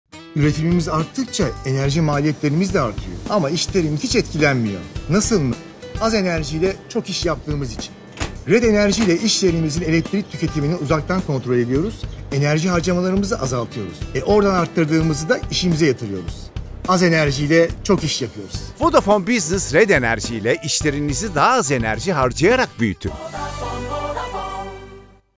Vodafone Business Red Enerji ile iş yerinizin elektrik tüketimini %30'a kadar azaltın!
Üretiminiz arttıkça maliyetiniz de artıyorsa, Vodafone Business Red Enerji ile tanışın. Vodafone Business Red Enerji ile iş yerinizin elektrik tüketimini uzaktan kontrol edin, %30’a varan enerji tasarrufu sağlayarak işlerinizi daha az enerji harcayarak büyütün.